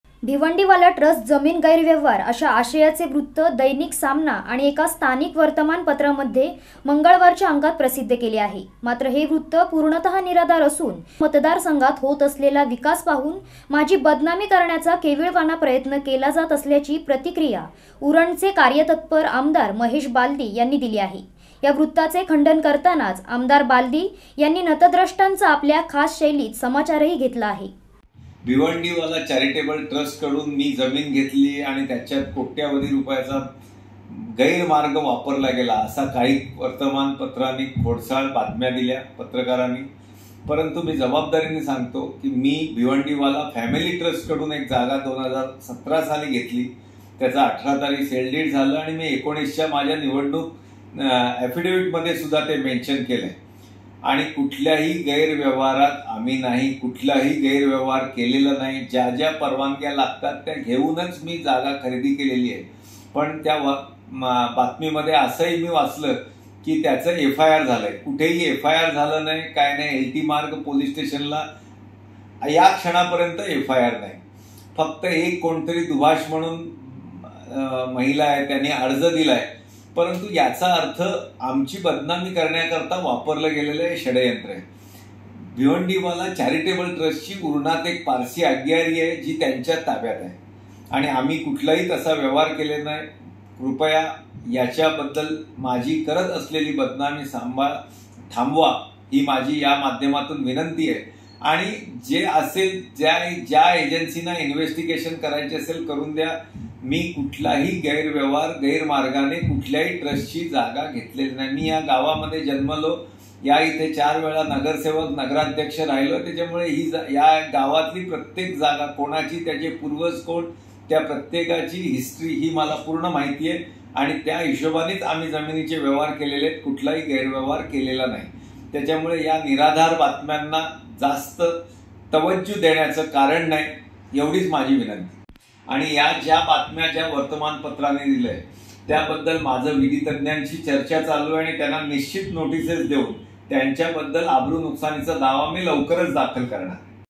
भिवंडीवाला ट्रस्ट जमीन गैरव्यवहाराचे वृत्त निराधार !
आमदार महेश बालदी यांनी नतद्रष्टांचा घेतला समाचार !